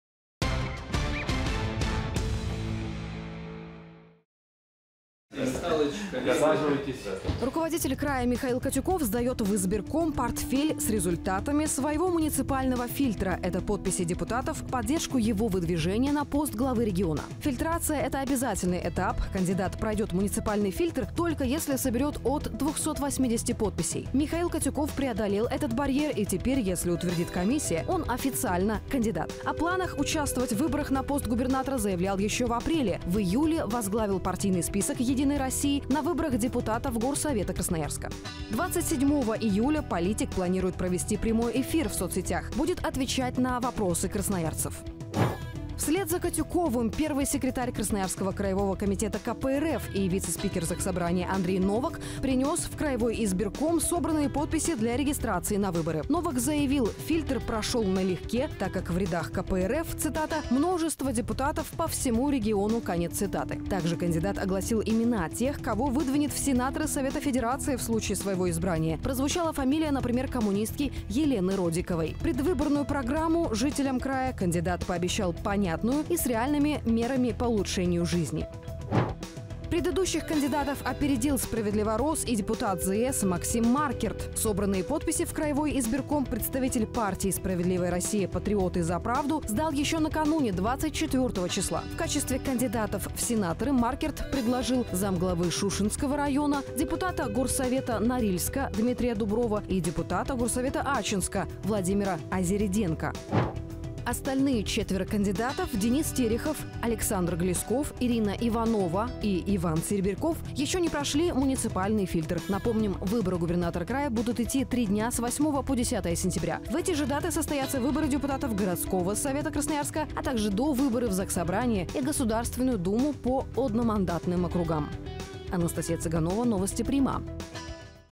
Михаил Котюков отнес в избирком подписи депутатов
Врио губернатора Михаил Котюков и глава краевых коммунистов Андрей Новак сегодня отнесли в избирком подписи депутатов, необходимые для регистрации на губернаторских выборах. Но их на один день опередил представитель партии «Справедливая Россия» Максим Маркерт, подробнее о предвыборной гонке прямо сейчас.

На фото руководитель края Михаил Котюков сдаёт в избирком портфель с результатами своего муниципального фильтра. Это подписи депутатов в поддержку его выдвижения на пост главы региона. Фильтрация — процесс необходимый, кандидат пройдёт муниципальный фильтр, только если соберет от 280 подписей. Михаил Котюков преодолел этот барьер, и теперь, если утвердит комиссия, он официальный кандидат. О планах участвовать в выборах на пост губернатора заявлял ещё в апреле. В июле возглавил партийный список «Единой России» на выборах депутатов горсовета Красноярска. 27-го июля политик планирует провести прямой эфир в соц.сетях — будет отвечать на вопросы красноярцев.

Вслед за Котюковым, первый секретарь Красноярского краевого Комитета КПРФ и вице-спикер Заксобрания Андрей Новак подал в краевой избирком собранные подписи для регистрации на выборы. Новак заявил, фильтр прошёл налегке, так как в рядах КПРФ цитата «множество депутатов по всему региону», конец цитаты. Так же кандидат огласил имена тех, кого выдвинет в сенаторы Совета Федерации в случае своего избрания, прозвучала фамилия, например, коммунистки Елены Родиковой. Предвыборную программу жителям края кандидат пообещал понятную и с реальными мерами по улучшению жизни.

Предыдущих кандидатов опередил справедливоросс и депутат ЗС Максим Маркерт, собранные подписи в краевой избирком представитель партии «Справедливая Россия — Патриоты — За правду» сдал ещё накануне, 24-го числа. В качестве кандидатов в сенаторы Маркерт предложил: замглавы Шушенского района, депутата \горсовета Норильска Дмитрия Дуброва и депутата горсовета Ачинска Владимира Озереденко.

Остальные четверо кандидатов — Денис Терехов, Ирина Иванова, Александр Глисков, Иван Серебряков — ещё не прошли муниципальный фильтр.
Напомним, выборы губернатора края будут идти три дня — с 8 по 10 сентября. В эти же даты состоятся выборы депутатов городского Совета Красноярска, а также довыборы в Заксобрание и Государственную Думу по одномандатным округам.